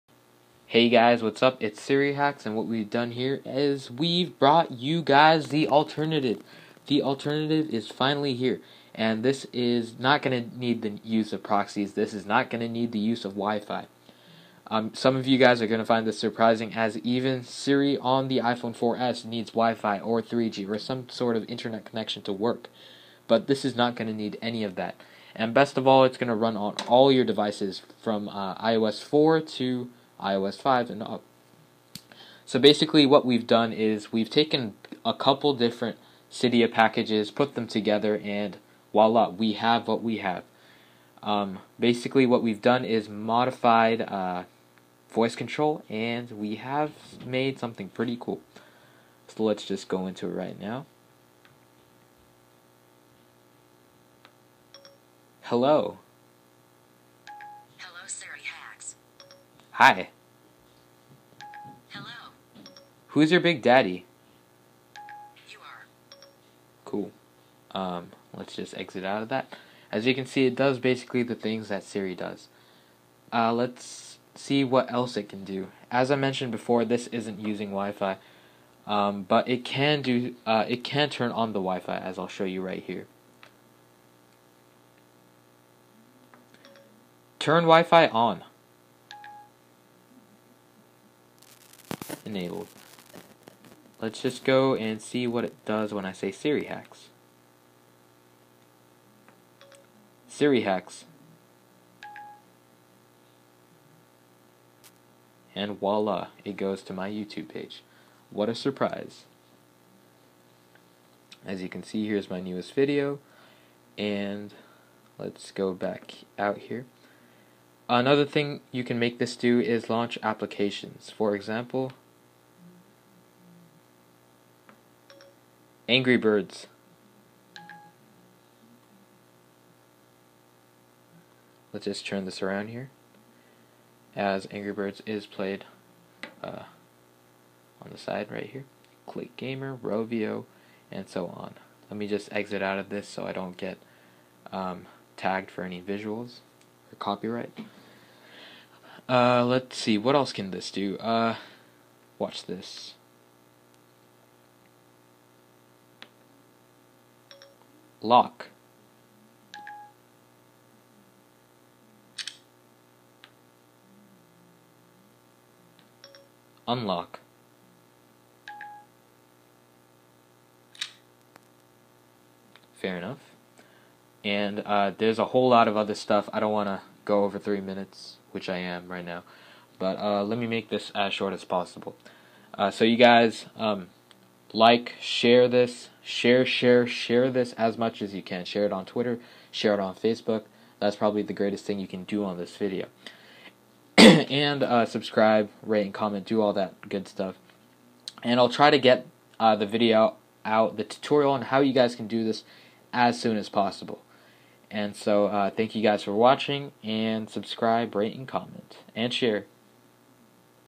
100% Working Siri Alternative finally available on jailbroken devices!
Siri Alternative finally available on jailbroken devices! Works by modifying voice control. No proxy required! I'll be posting the video on how to do this as soon as possible (probably 1-2 days). The best thing you can do for me is share. Share on facebook and twitter!